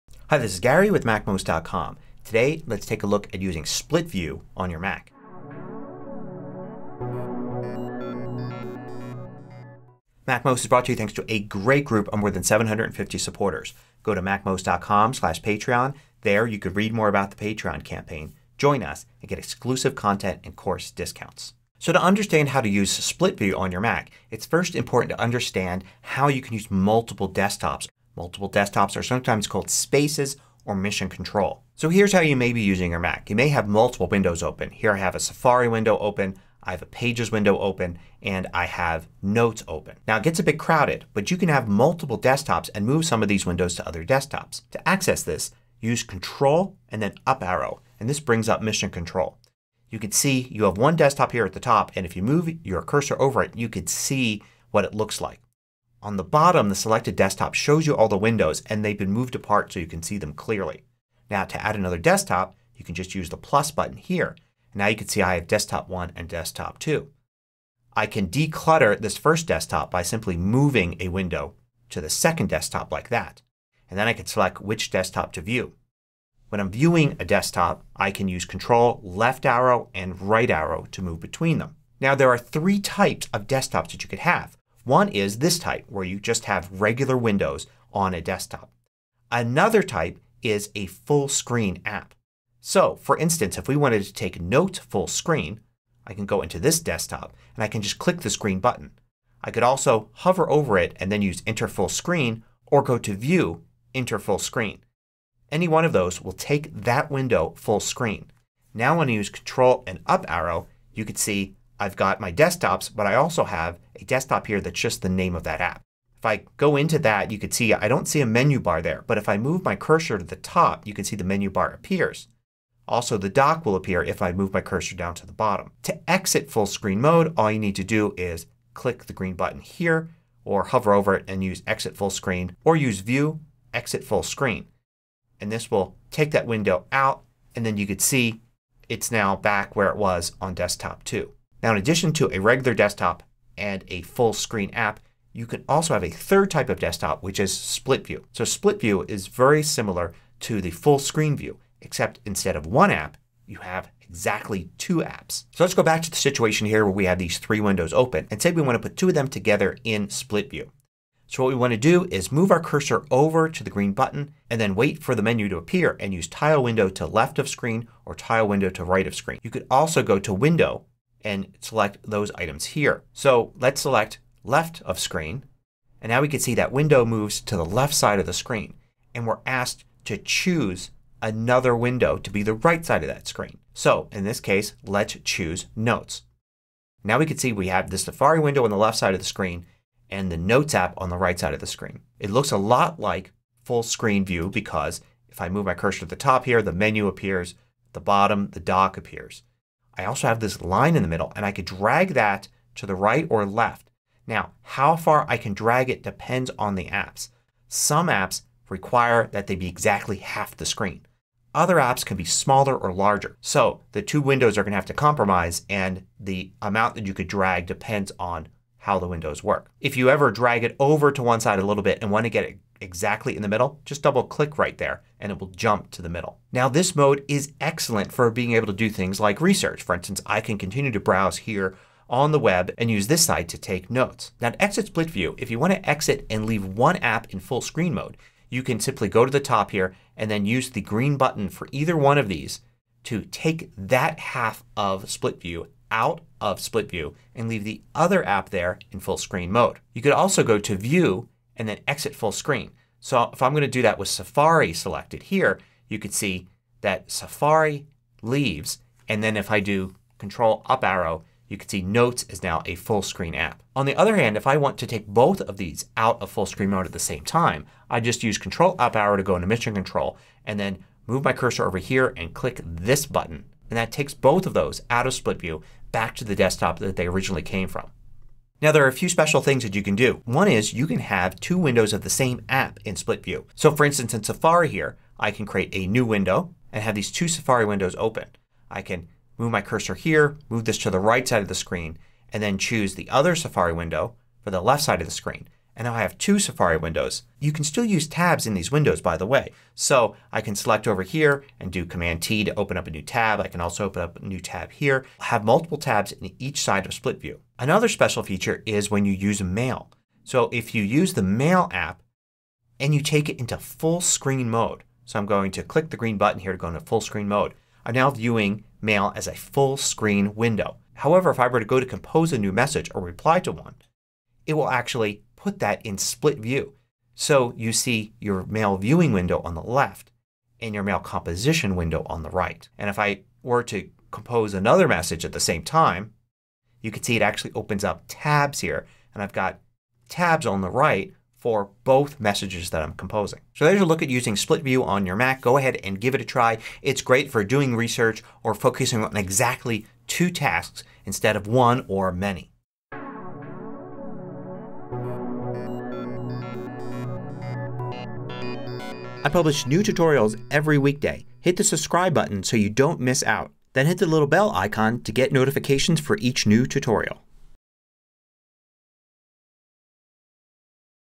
Mac Basics: How To Use Split View
Split View allows you to place two application windows side-by-side on your Mac with no other windows or elements. You can use this as an alternative to arranging windows on your desktop or using a single window in Full Screen mode.